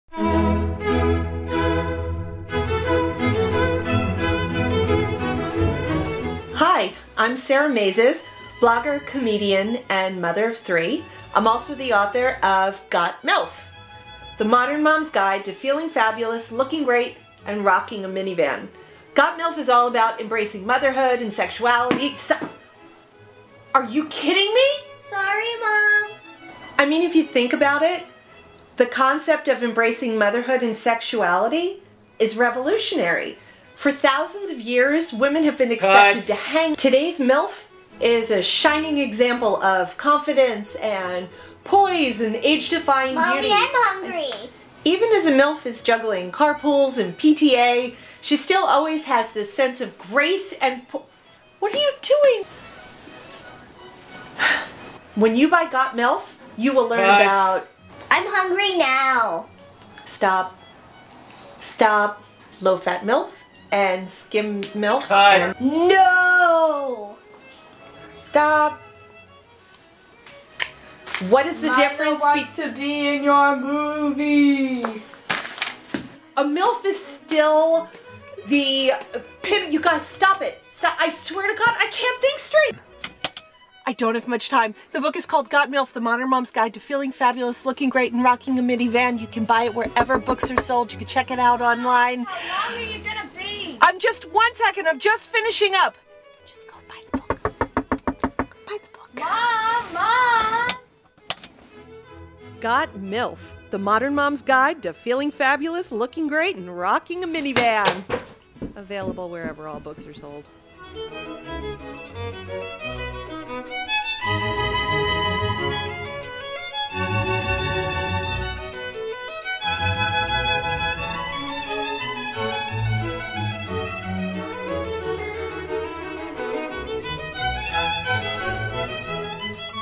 Got Milf? Funny Book Trailer - Sarah Maizes
This is the official book trailer for "Got Milf? The Modern Mom's Guide to Feeling Fabulous, Looking Great and Rocking a Minivan" by stand-up comedian and author Sarah Maizes